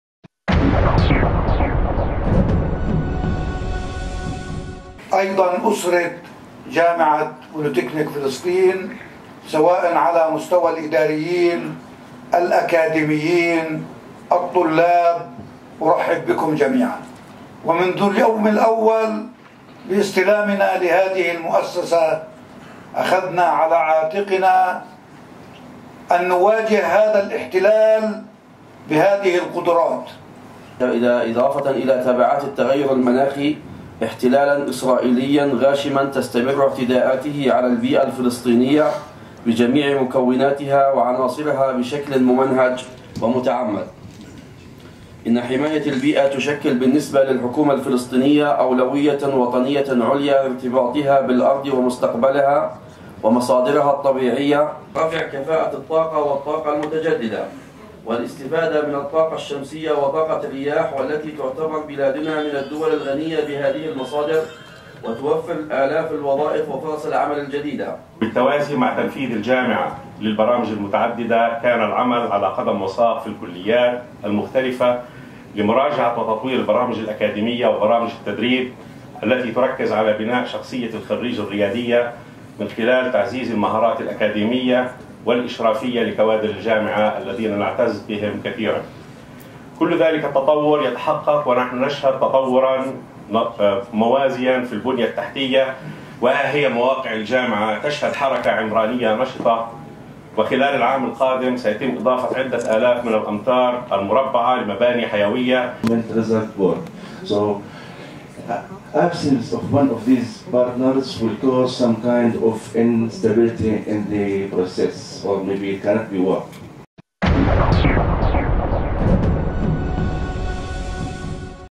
جامعة بوليتكنك فلسطين تحتفل في اليوم العلمي الختامي للشراكة الثلاثية
تصوير: أمير أبو مرخية
مونتاج: شادي سلهب